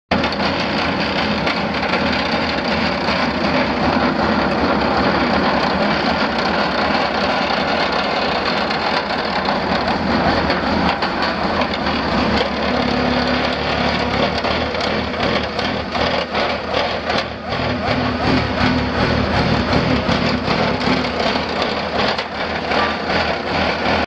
Wolverine 73"W skid steer smooth drum vibratory roller at auction
Wolverine  73"W skid steer smooth drum vibratory roller
 Serial: 21HKNY040-47
It's located in Spring Valley, WI, and we're selling it to the highest bidder regardless of price.